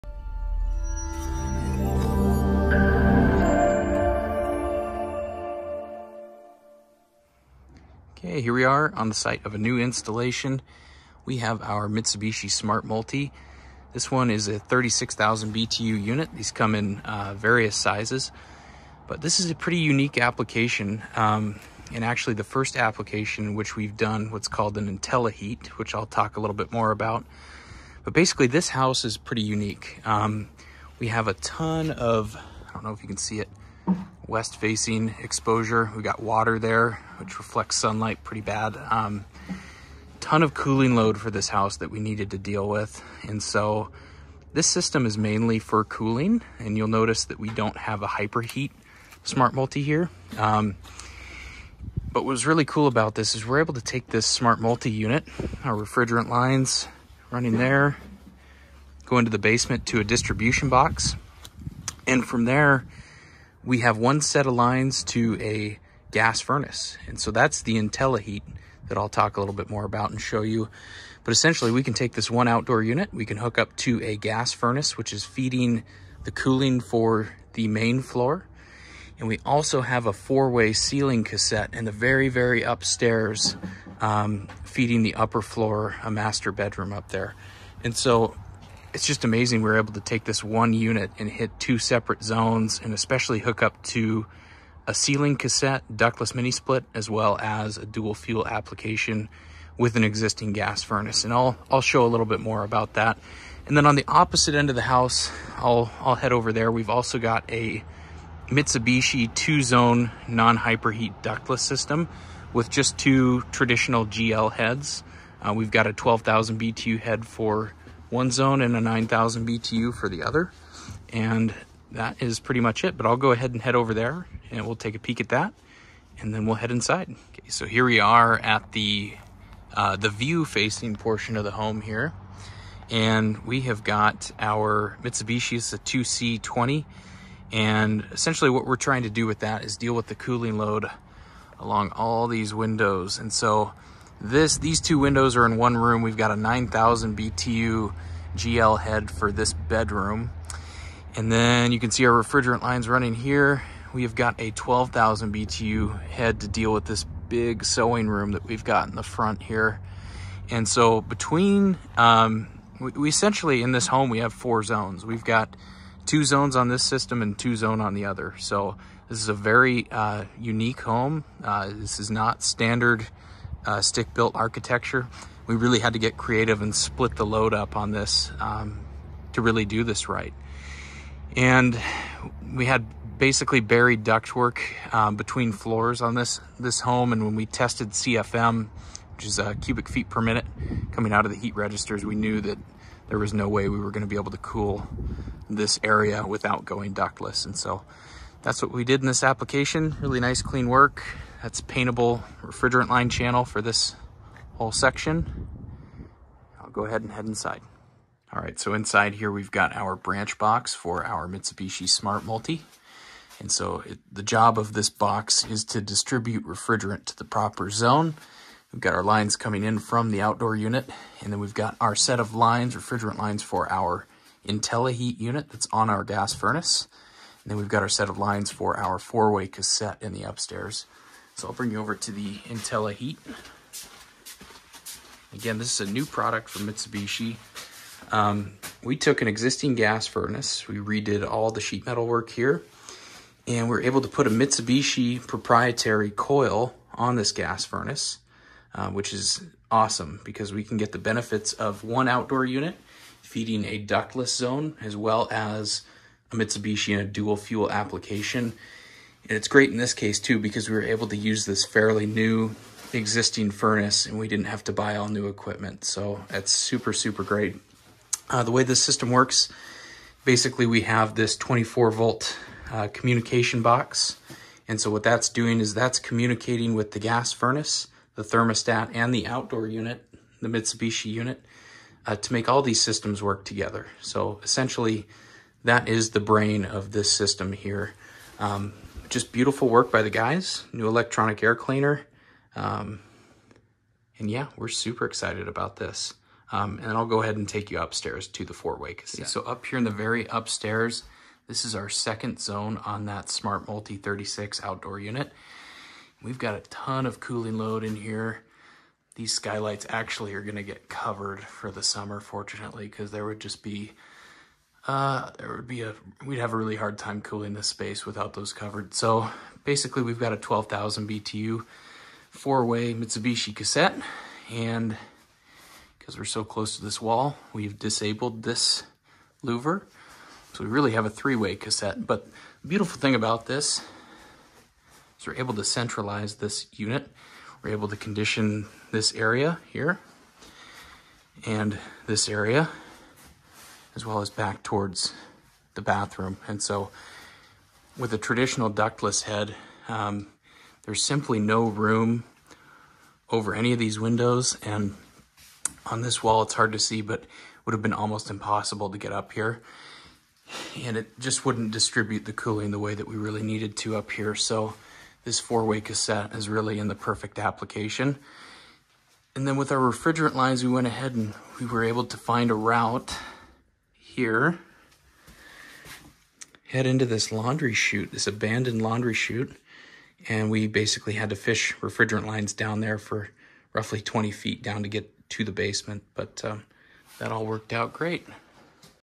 Mitsubishi Smart Multi with Intelli-HEAT Dual Fuel Heat Pump AND Mitsubishi 4-Way ceiling cassette
In this video we show the capabilities of a Mitsubishi Smart Multi. We are able to pair one outdoor unit with minisplit heads, as well as Mitsubishi's Dual Fuel Intelli-HEAT Coil that we are able to add to an existing or new gas furnace. This system is going to be HUGE in the PNW as it allows us extreme flexibility and consumer savings as we transition to heat pump systems from existing gas systems.